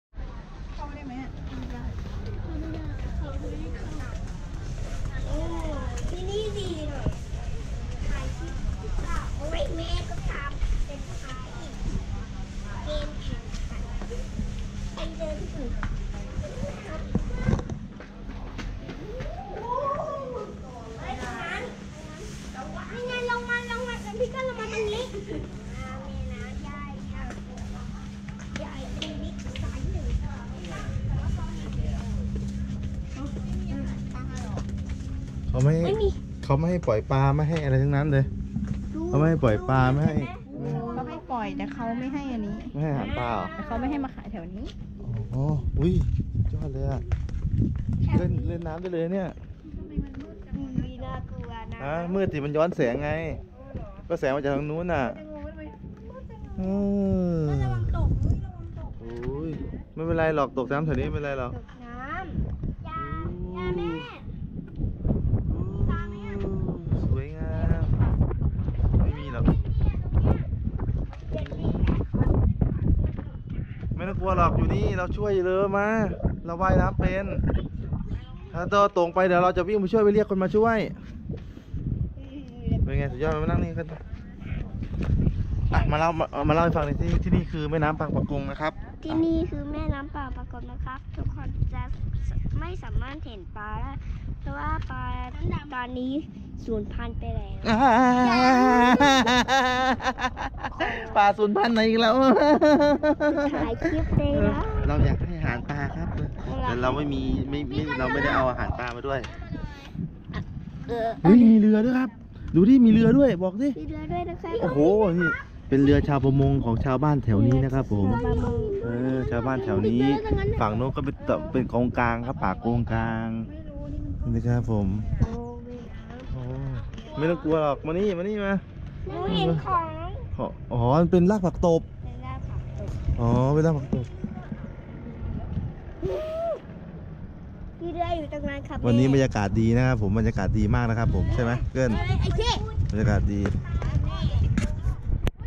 ตะวันกับพี่เกิลพาไปดูแม่น้ำบางปะกงหลังวัดโสธรวรารามวรวิหาร
อาโปยังไม่มาเกิดเลย ถ่ายตอนปี 62 จะหาโอกาสไปอีก ไม่ได้ไปนานมากๆ